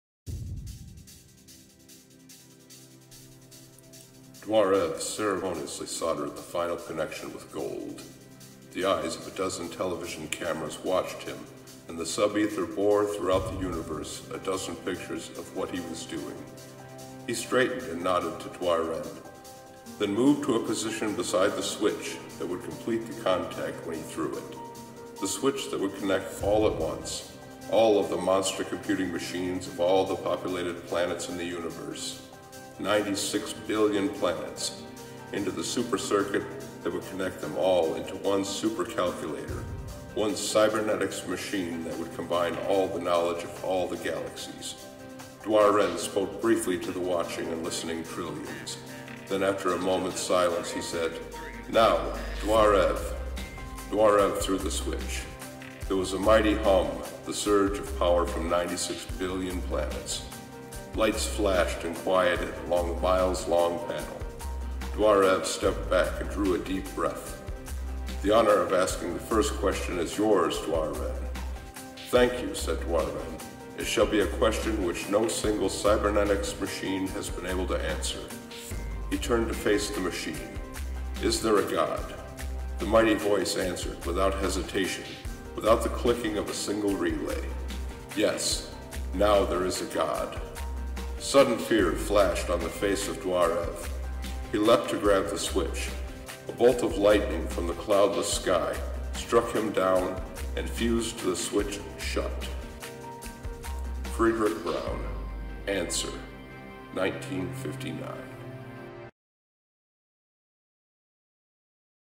Answer, Fredric Brown 1959
My audio reading of Fredric Brown's short story Answer from 1959 as a teaser for the upcoming podcast on April 10th, 2021 regarding Artificial Intelligence.